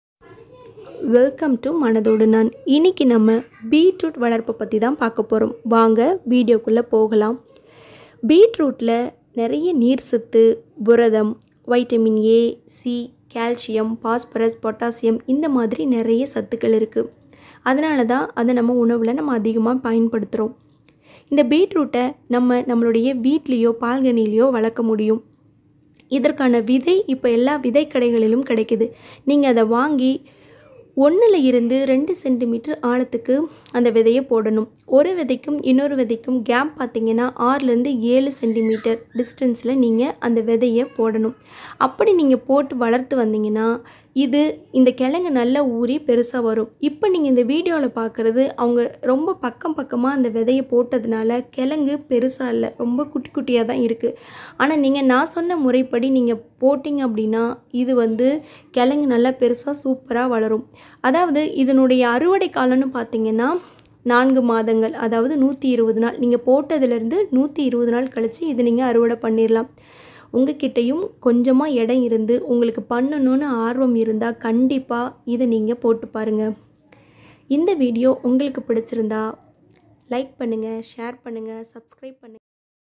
# Beetroot cultivation# beetroot growing/ beetroot valarpu Murai/ beetroot harvesting
Beetroot cultivation
The beetroot is the taproot portion of a beet plant, usually known in Canada and the USA as beets while the vegetable is referred to as beetroot in British English, and also known as the table beet, garden beet, red beet, dinner beet or golden beet.